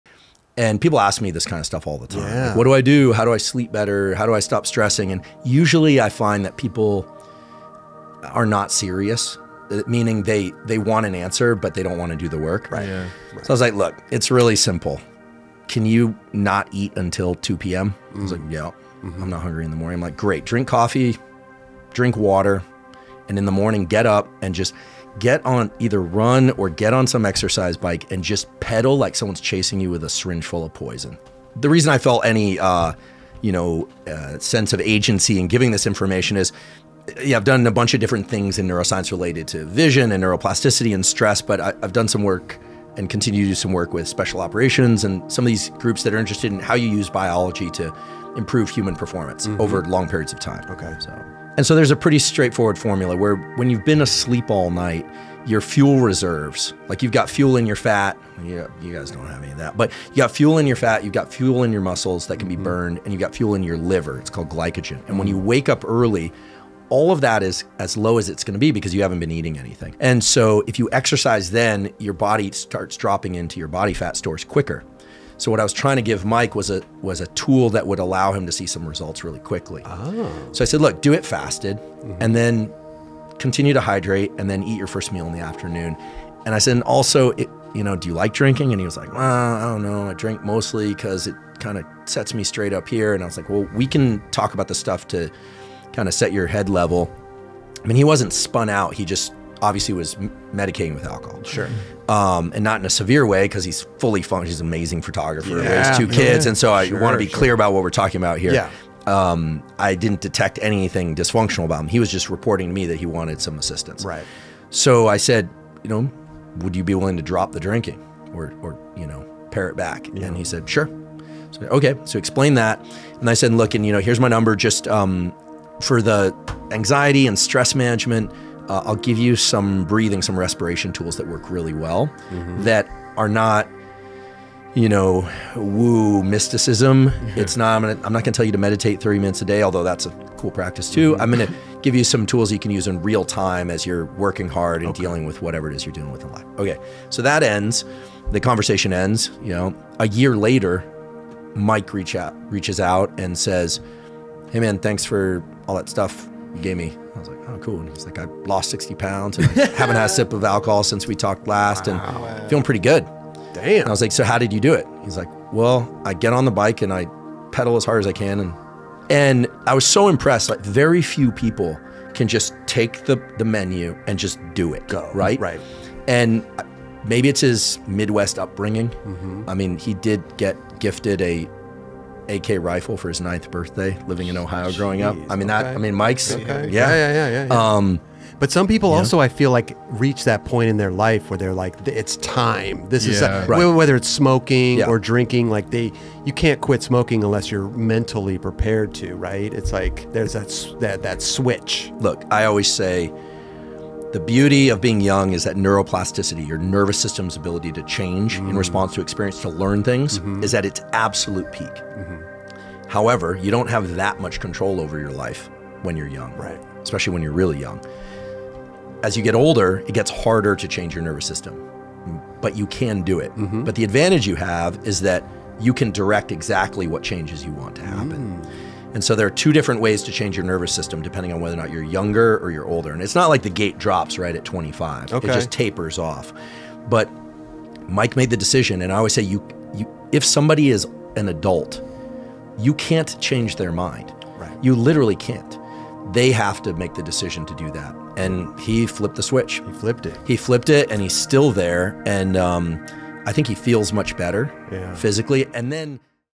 NEUROSCIENTIST: How To Lose Weight Fast | Andrew Huberman
Neuroscientist Andrew Huberman reveals the truth about achieving fitness goals such as getting in shape and losing weight, and shares insights on how to actually attain them.